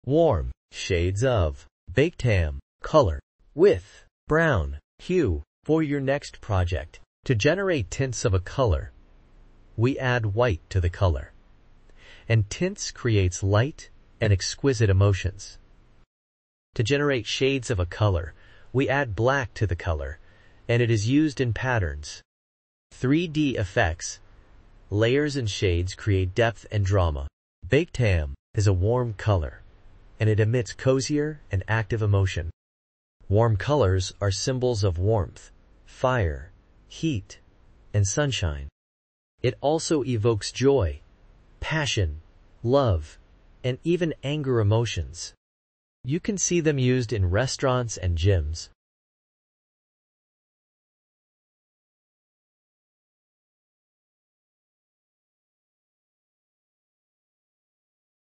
Shades & Tints of Baked Ham color eec8bc A Warm Brown color
Shades of Baked Ham color eec8bc Warm Shades of Baked Ham color with Brown hue for your project!

Shades
Hex d3b1a7

Tints
Hex efcec3

Baked Ham is a warm color, and it emits cozier and active emotion. Warm colors are symbols of warmth, fire, heat, and sunshine. It also evokes joy, passion, love, and even anger emotions. You can see them used in Restaurants and Gyms.